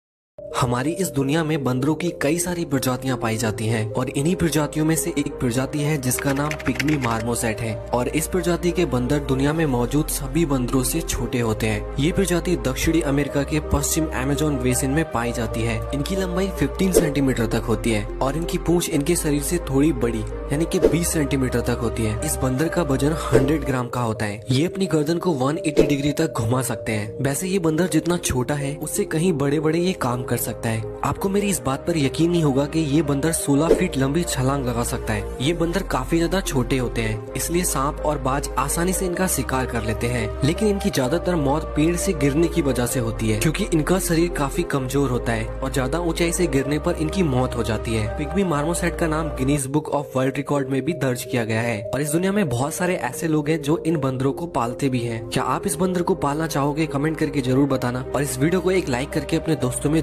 pygmy marmoset एक ऐसा बंदर जिसने बनाया हे।world record
the monkey species those are make world record in guinness book of world record
this is the very smallest monkey in the world .

एक ऐसा बंदर का प्रजाति जिसने बना दिया है गिन्नेस बुक वर्ल्ड रिकॉर्ड ।क्यूंकि ये दुनिया का सबसे छोटा बंदर है।